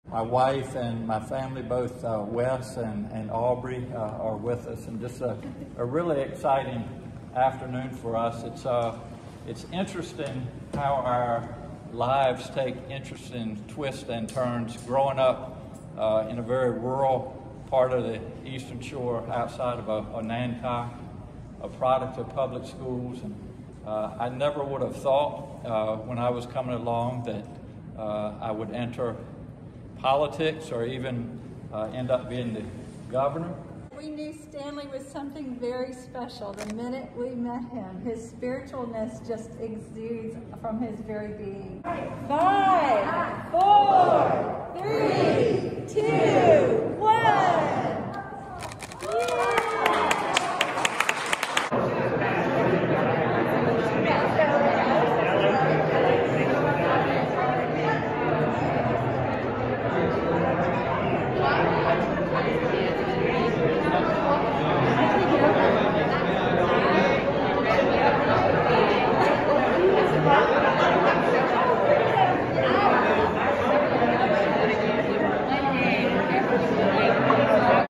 Northam’s Portait Unveiling
Northam's portrait, painted by artist Stanley Rayfield, was unveiled at the Virginia State Capitol in Richmond, Va., Tues., Jan. 11, 2022.  Video by Bob Brown/Times-Dispatch